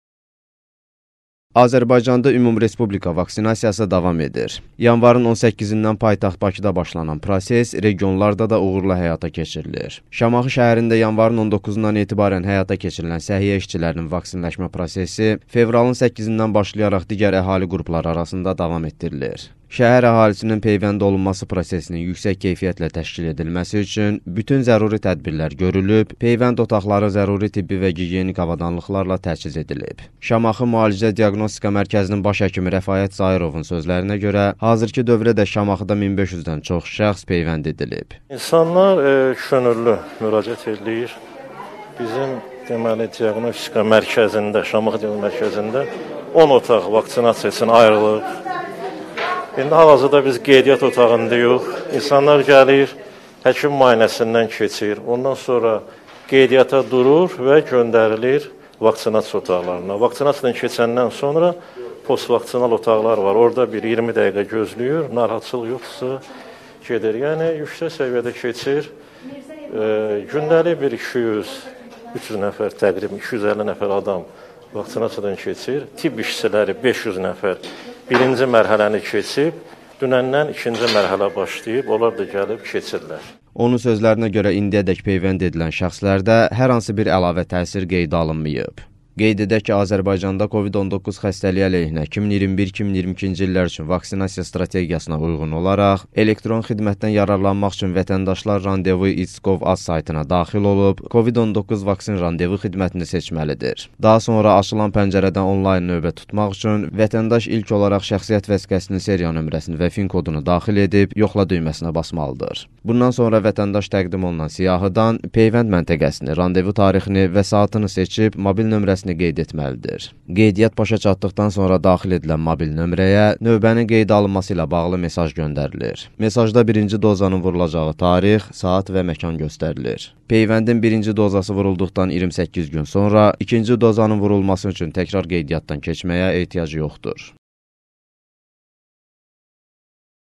Regionlarda vaksinasiya davam edir
Şamaxıda 1500-dən çox insan peyvənd edilib